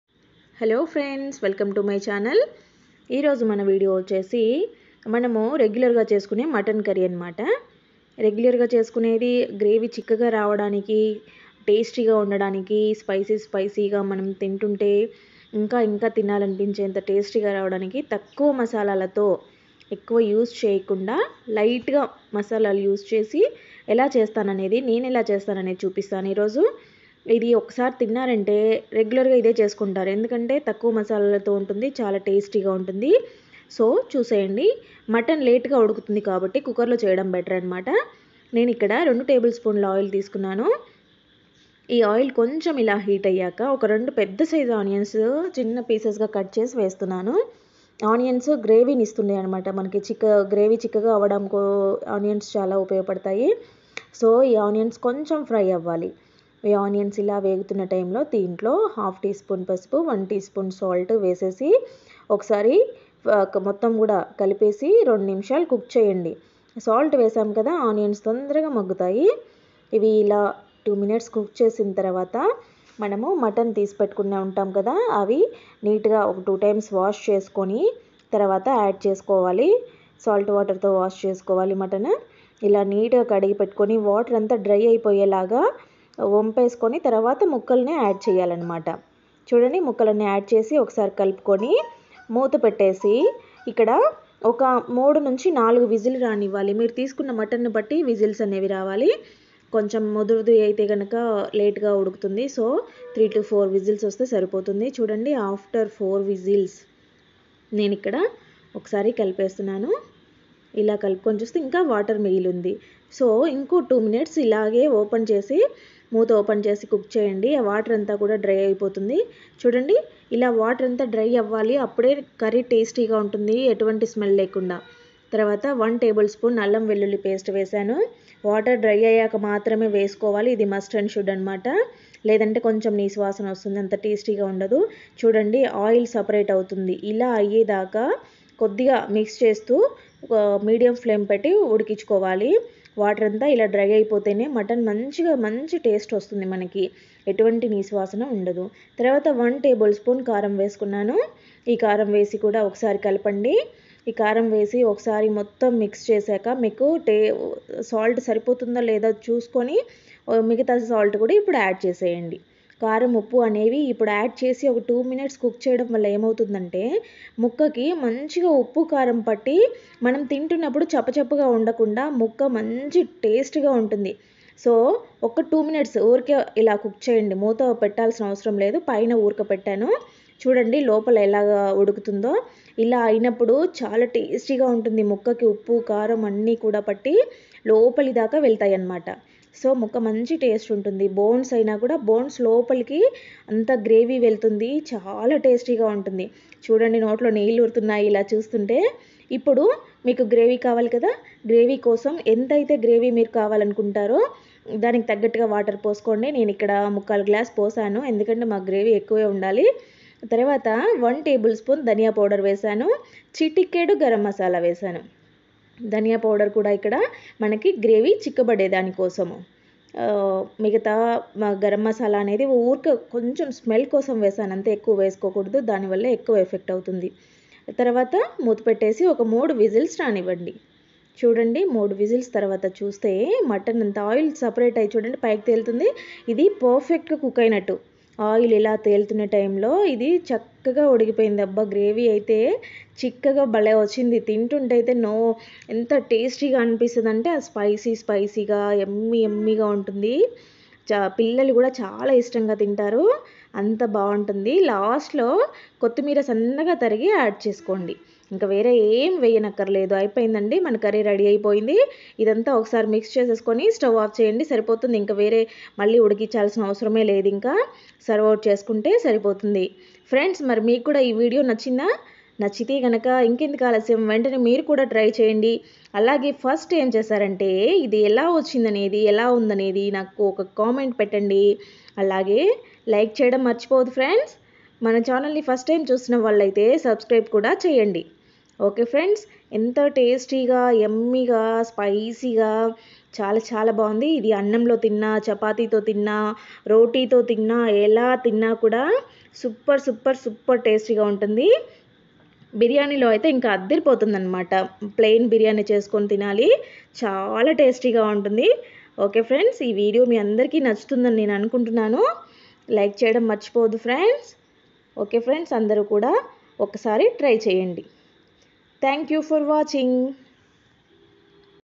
రెగ్యులర్ గా చేసుకునే మటన్ కర్రీ గ్రేవీ చిక్కగా రావాలంటే ఇలా👉చేయండి/తక్కువ మసాలాలతో రుచిగా😋 వుంటుంది
mutton curry in easy process and tasty